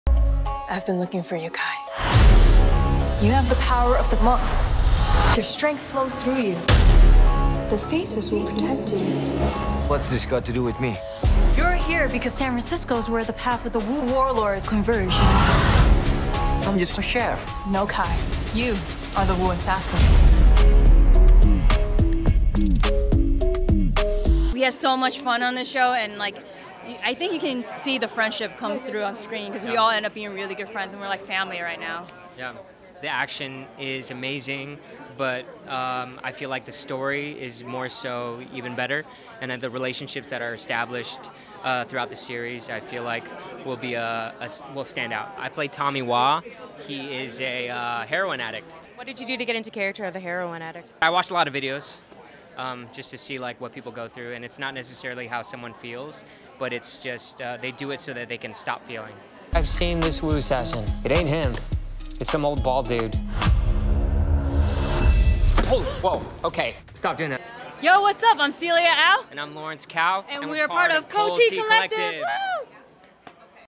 Wu Assassins - Celia Au & Lawrence Kao
We caught up with Celia Au and Lawrence Kao of the upcoming Netflix show, Wu Assassins and asked them about their characters and the show.